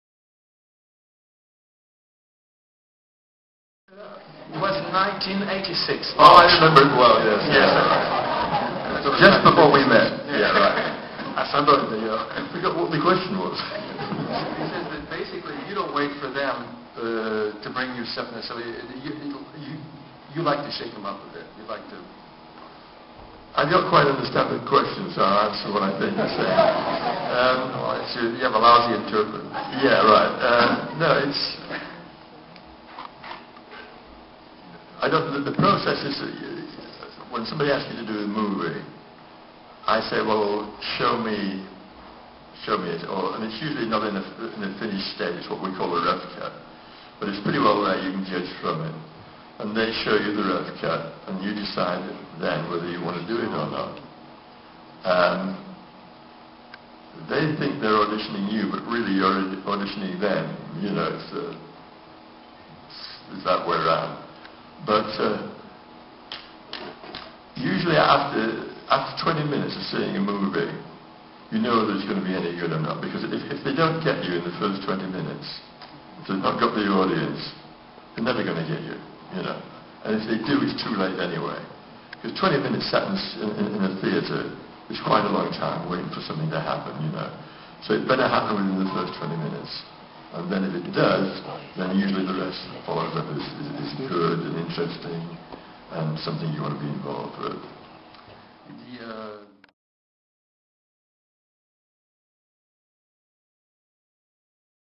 JOHN BARRY à Auxerre 2007
John Barry interviewé à Auxerre par Stéphane Lerouge et Bob Swaim lors du Festival Musique et Cinéma en 2007.